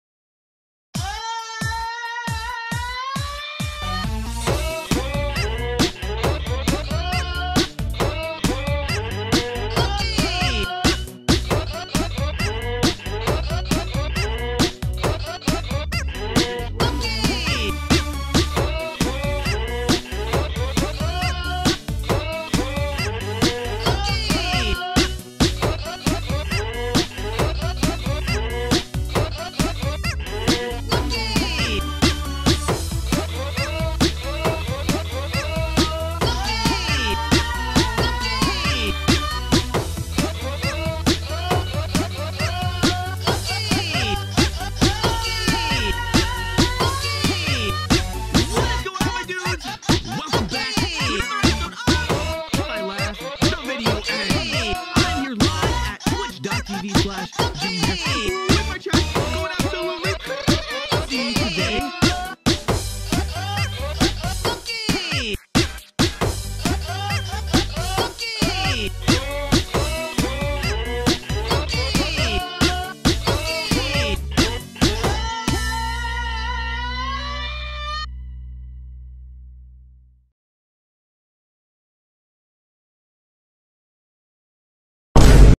Packing Monkeys (SHITPOST)
It isn't Wednesday right now, but I don't care. I'm just too eager to post this.
Also I really could have made the candy logo a monkey but I'm too fucking lazy to go back and change it.

The JimmyHere music mashup and custom sprites were by me.